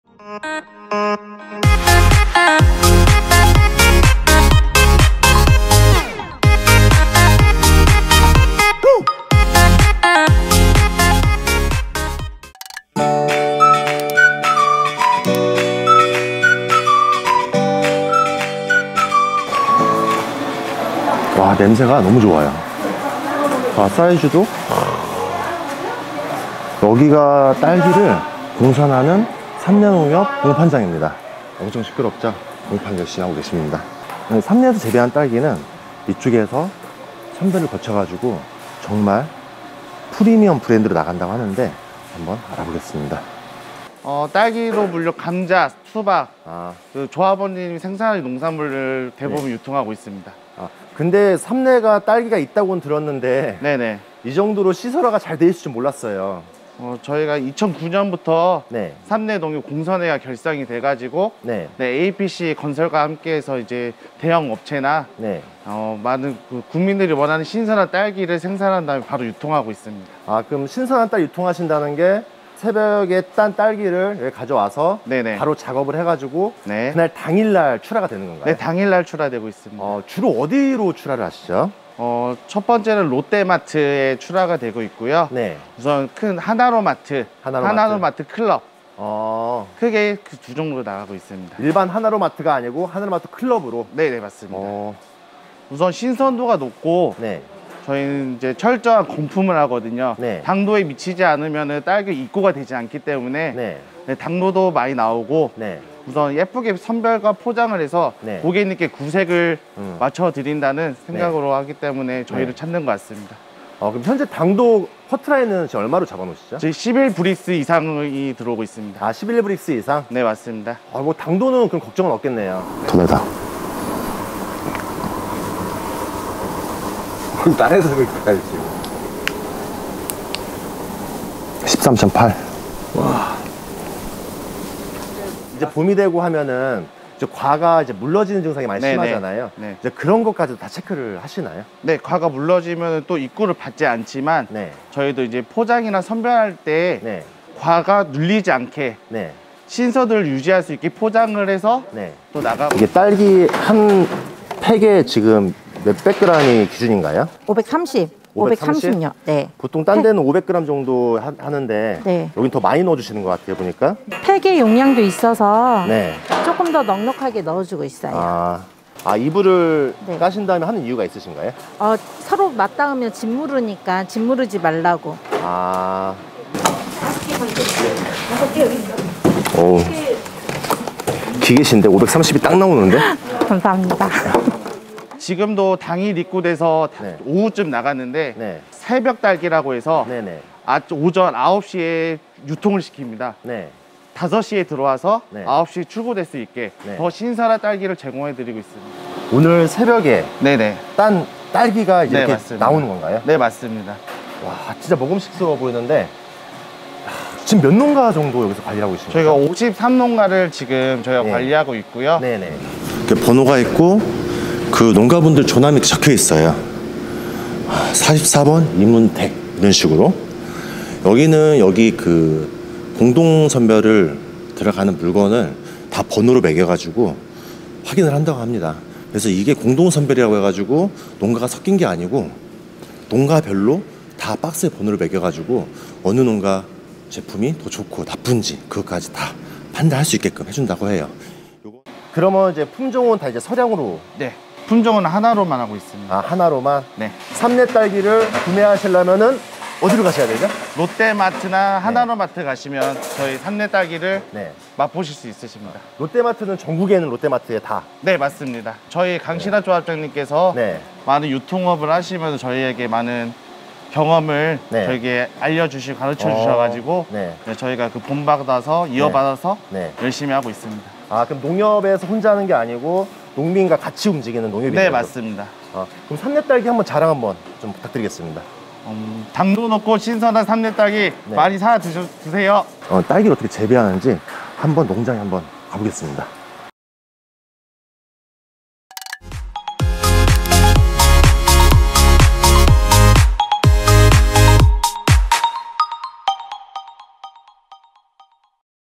딸기의 명품화 롯데마트, 하나로마트클럽에만 공급하는 삼례딸기 삼례농협산지유통센터 딸기의 명품화 공장 Strawberry Luxury Factory
명품 딸기를 찾아서
삼례딸기는 명품딸기를 고집합니다.

선진한국 농업의 시작은 언제나 이팜티비로 부터